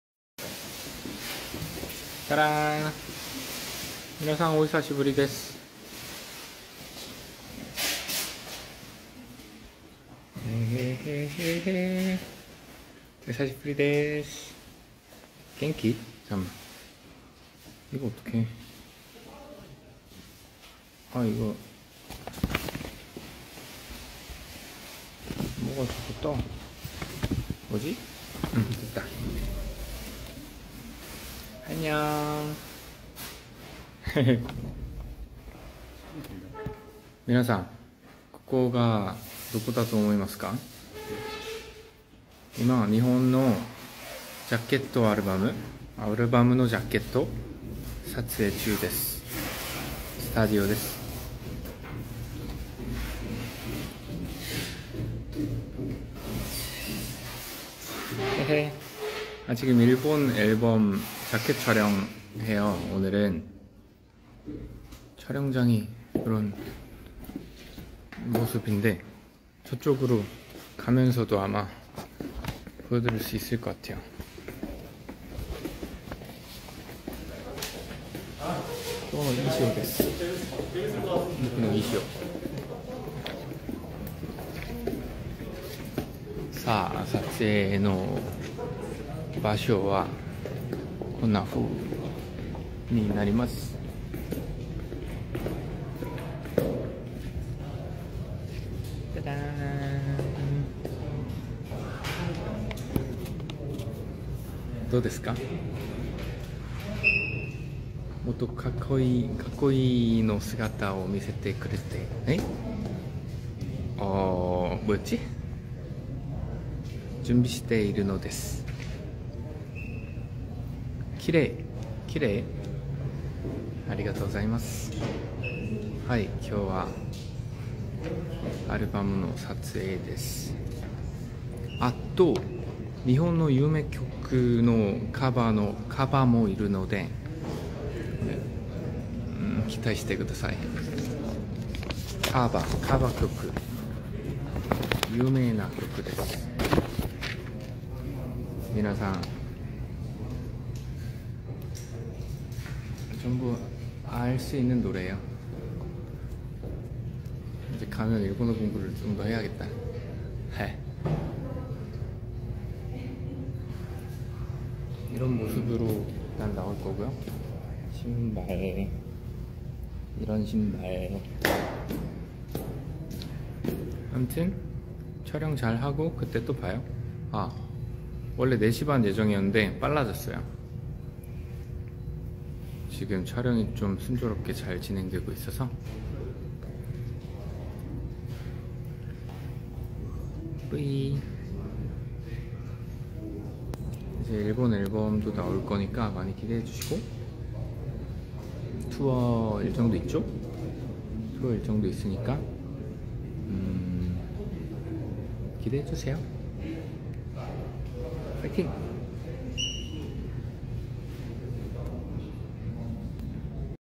Onew IG Live 220428 via SHINee Japan IG
‘Everyone long time no see!Long time no see~ how are you? Everyone, where do you think am I? I’m shooting the jacket of my Japanese album right now. At the studio.’

‘The shooting set up looks like this. Tada~ how is it? I’m preparing to show an even cooler side of me. (comment ‘beautiful’) beautiful? Thank you! Yes, I’m shooting an album jacket today’

‘Also, there’ll be my cover of a popular Japanese song on it (the new album), please look forward to it. A cover song… a popular song. Everyone, (starts talking in Korean). Fighting! 💪

trans: SPoet04